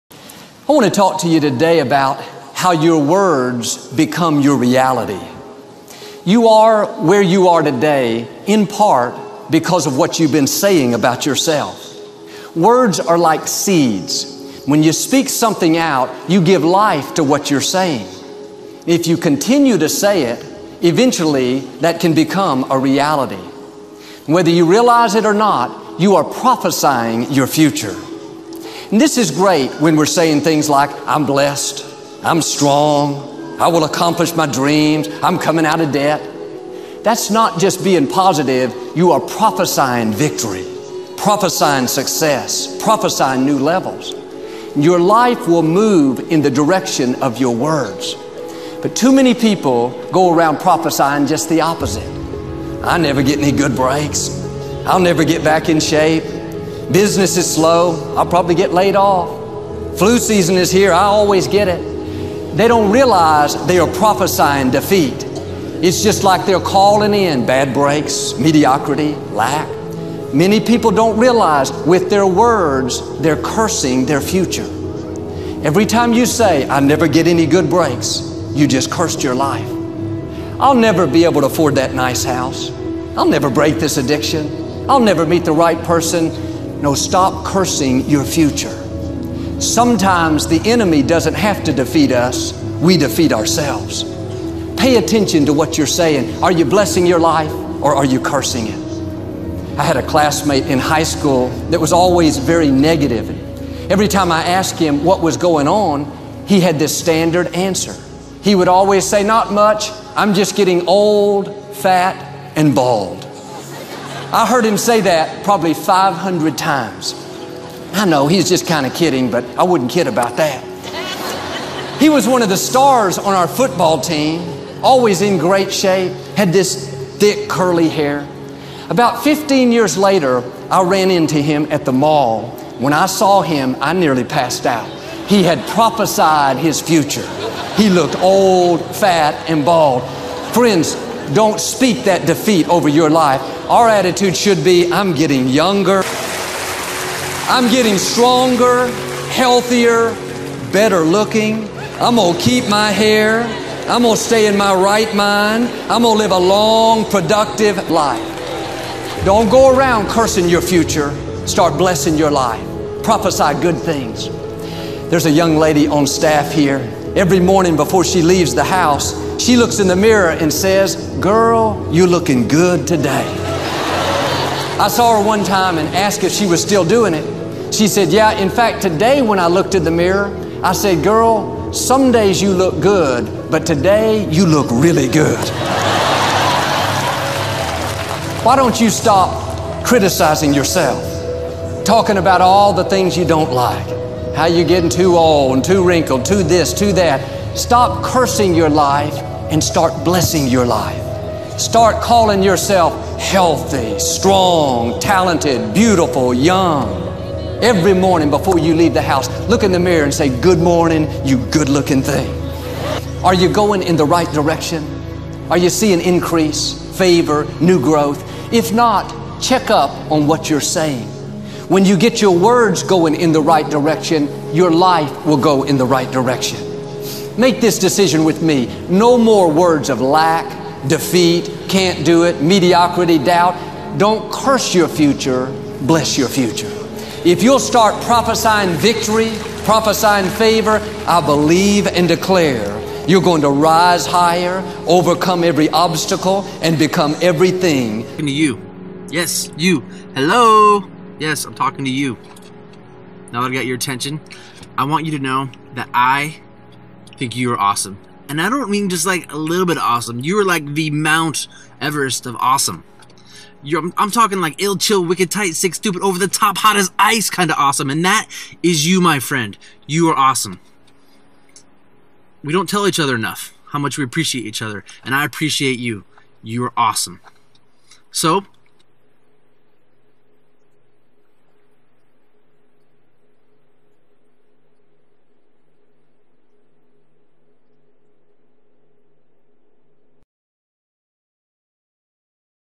YOUR WORDS BECOME YOUR REALITY
inspiration by joel osten on how to change your life and reality.